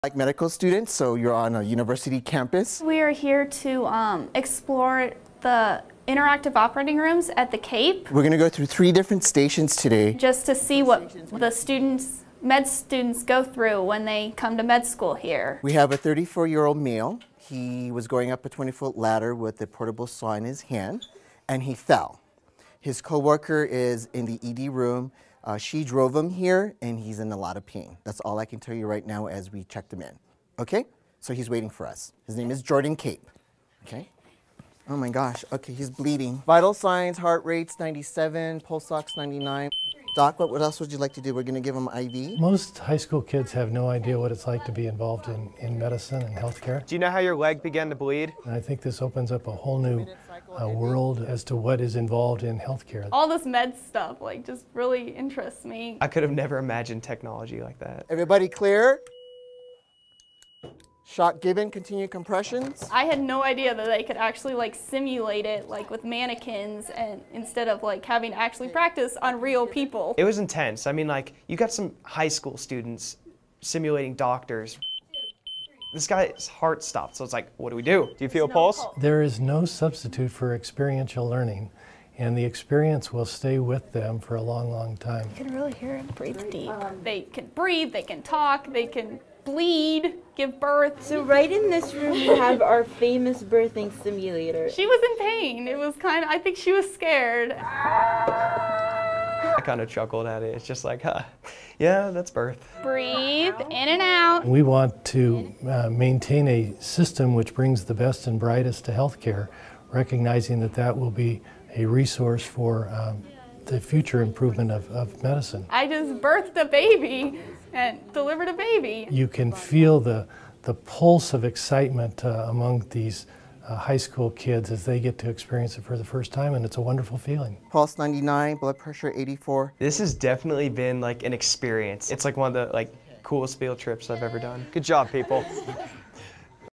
Students deliver fake baby
Not many high school students get a chance to help deliver a baby. And not many want to–except for a group of students from Chaparral High School in Parker.